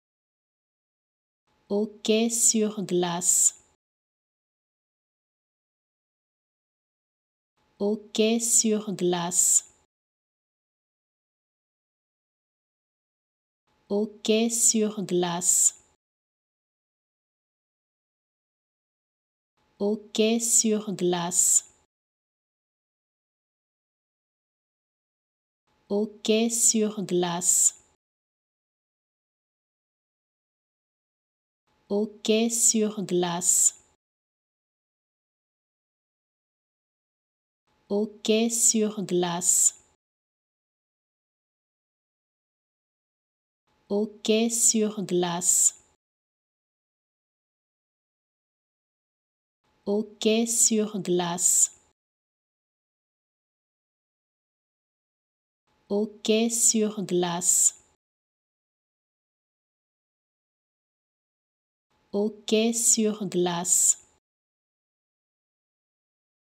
How to Pronounce ''Hockey sur glace'' Correctly in French
Learn how to say and properly pronounce ''Hockey sur glace'' in French with this free pronunciation tutorial. Apprenez à prononcer des mots en français grâce à nos tutoriels.